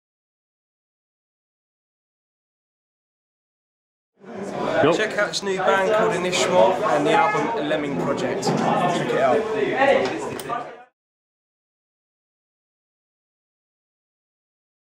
Andy James - Inishmore promo
A personal message from Andy James to all Inishmore fans out there!